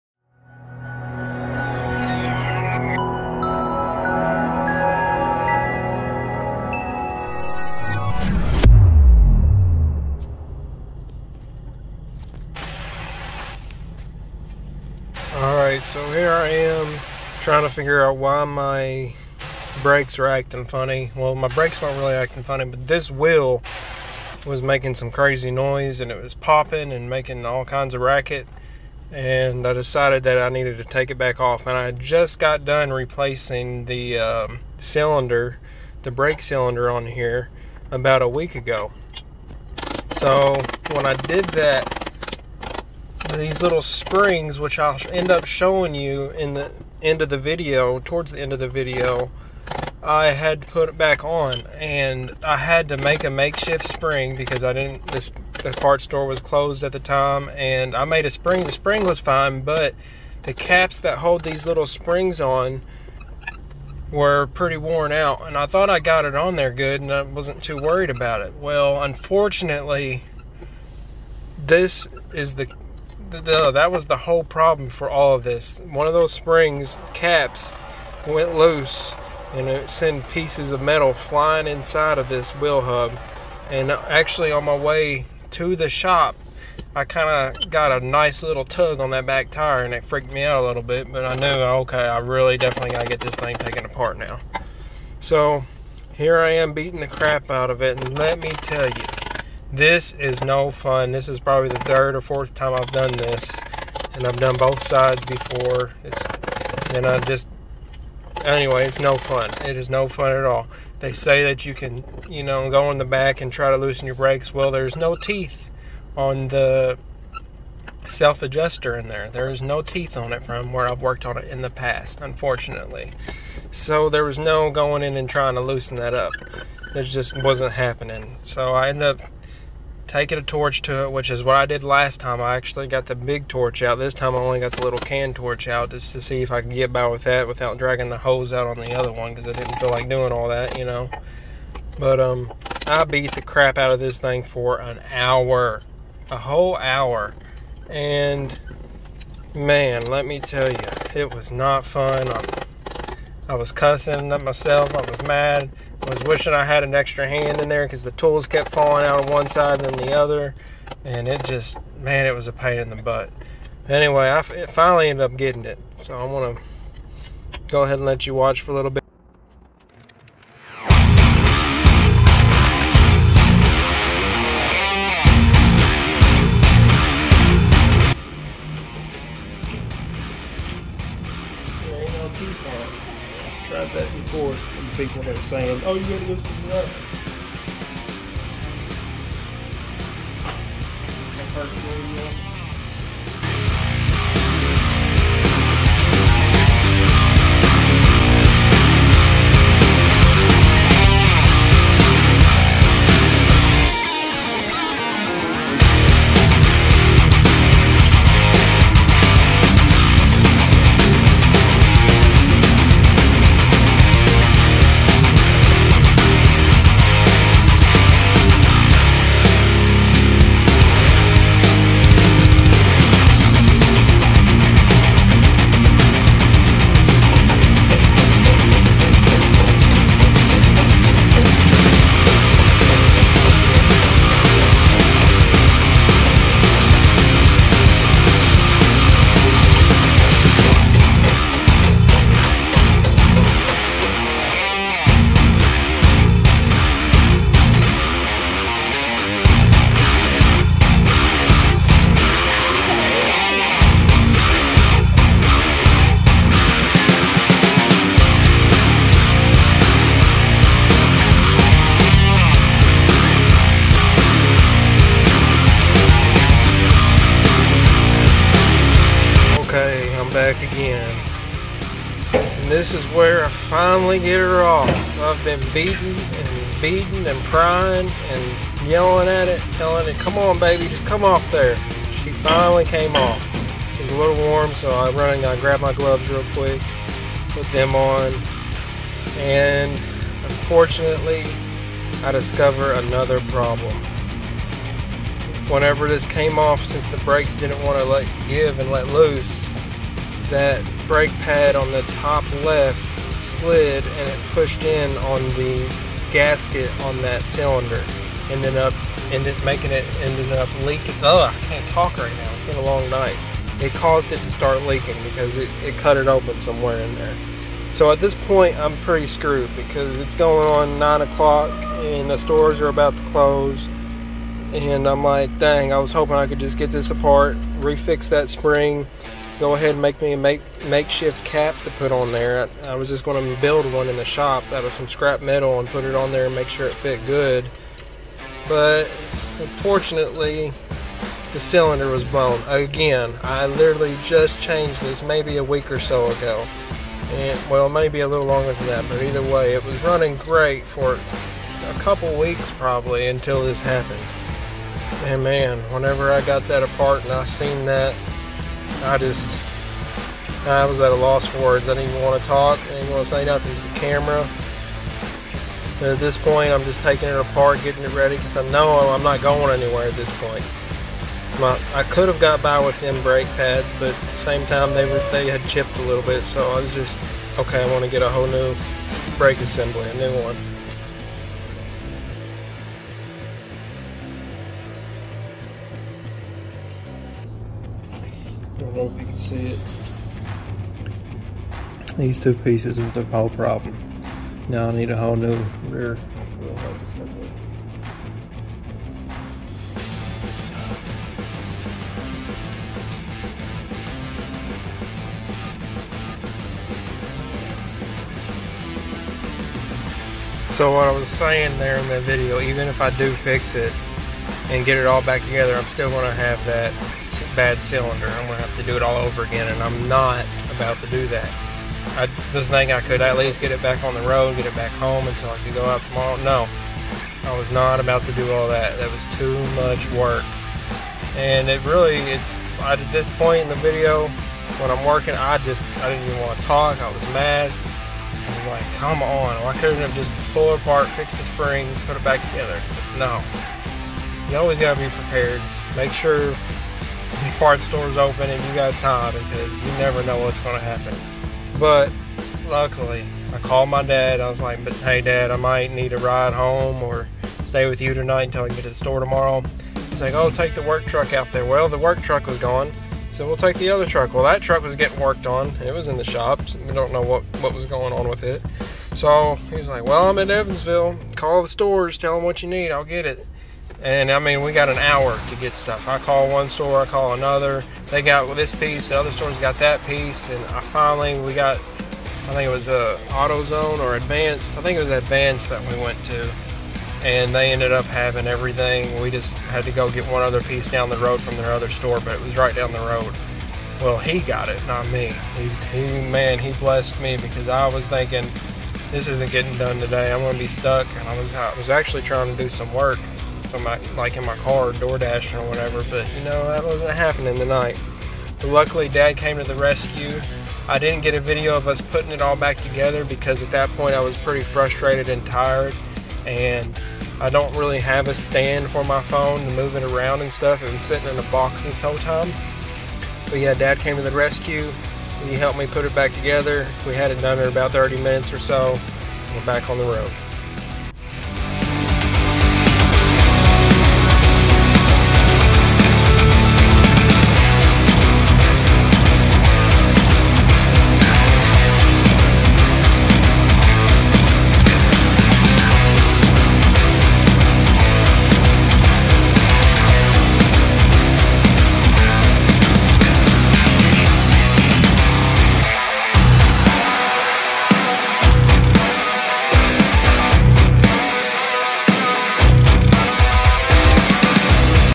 removing rear drum, 07 dodge caliber.
This video shows you how to remove the rear drum from a 2007 Dodge Caliber. A lot of videos make this seem easy, but it can be tricky, especially on older cars. If you're having a hard time, don't give up!